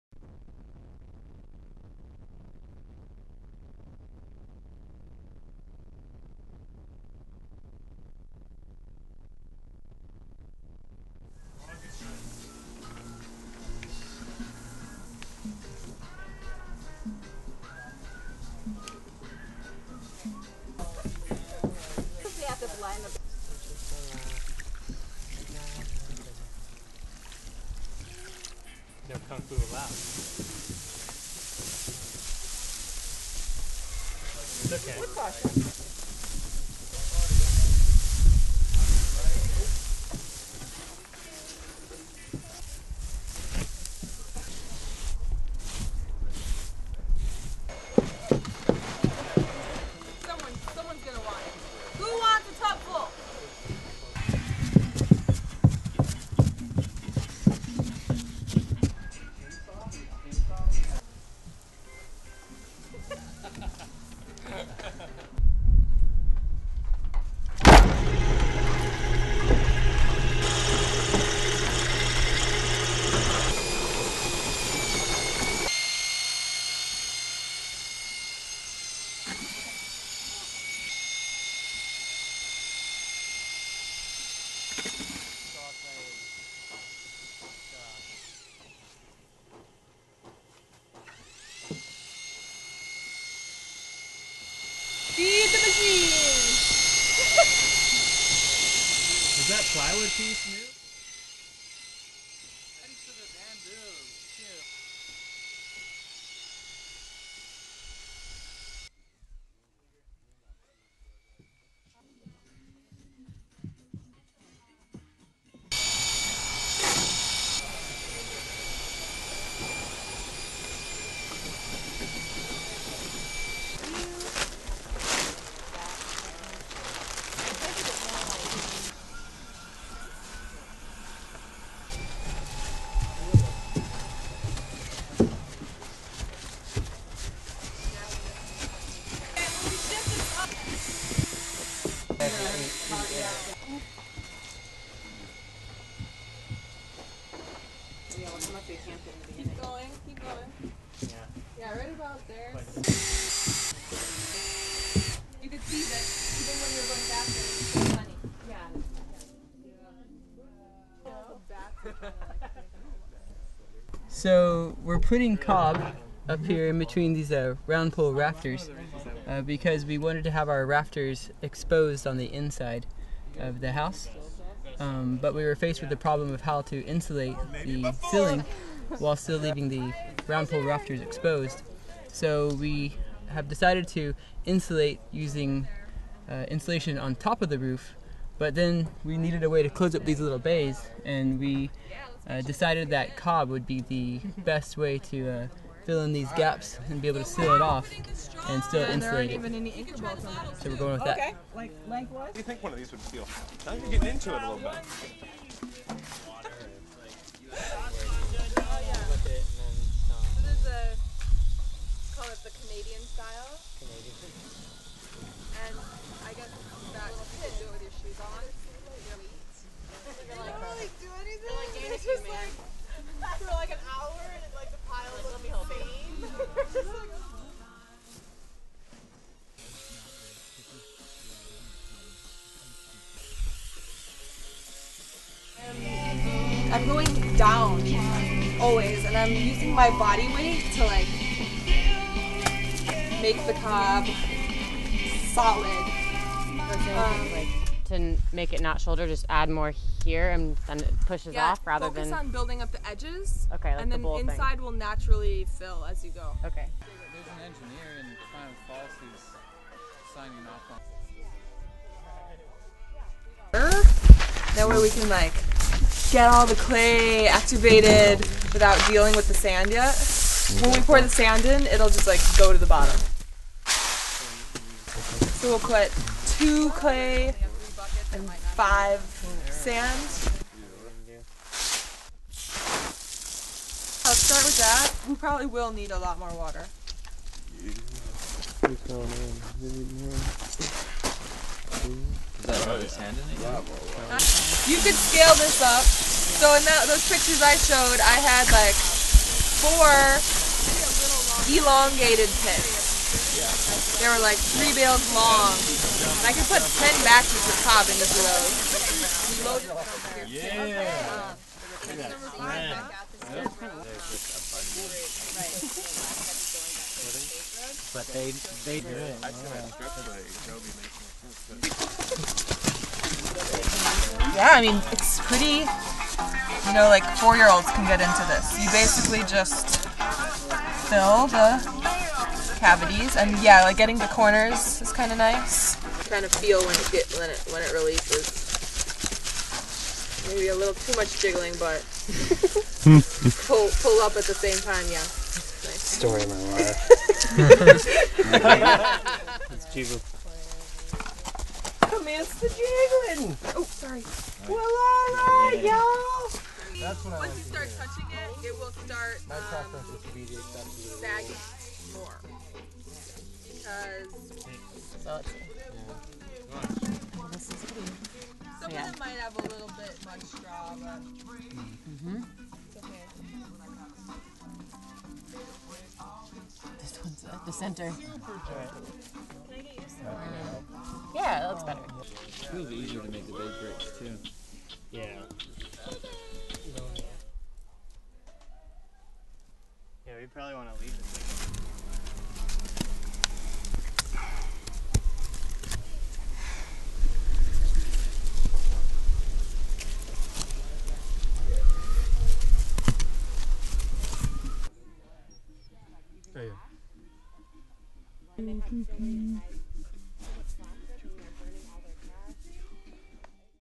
Sustainable Shelter Series: Wall Systems
Aprovecho Natural Building program reviews natural wall systems both those of insulation and those of mass.